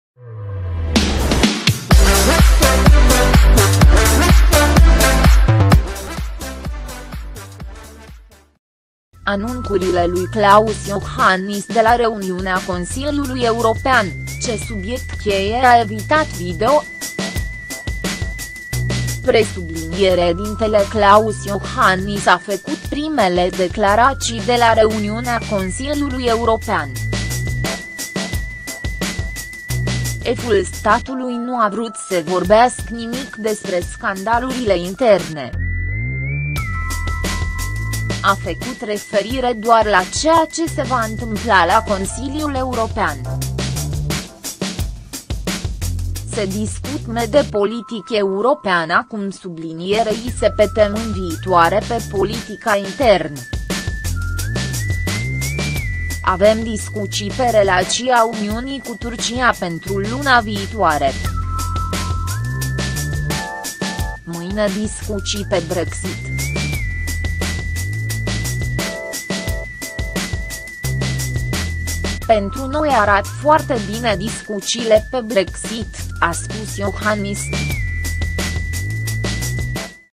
Anunţurile lui Klaus Iohannis de la reuniunea Consiliului European: Ce subiect-cheie
Anunţurile lui Klaus Iohannis de la reuniunea Consiliului European: Ce subiect-cheie a EVITAT / VIDEO